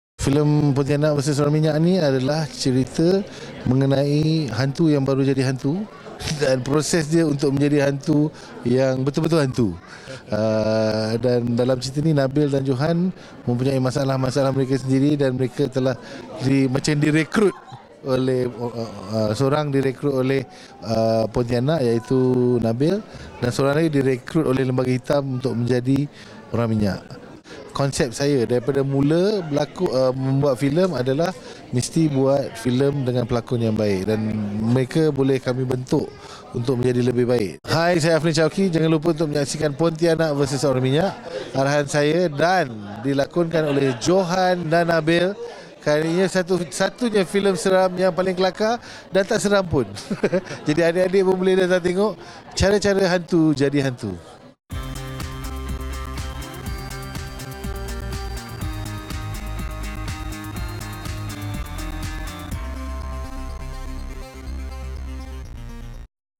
Majlis Pratonton Filem Pontianak VS Orang Minyak 8/11/2012
Majlis Pratonton Filem Pontianak VS Orang Minyak arahan Afdlin Shauki yang telah diadakan di Cathay E@Curve,Damansara pada 8 November 2012.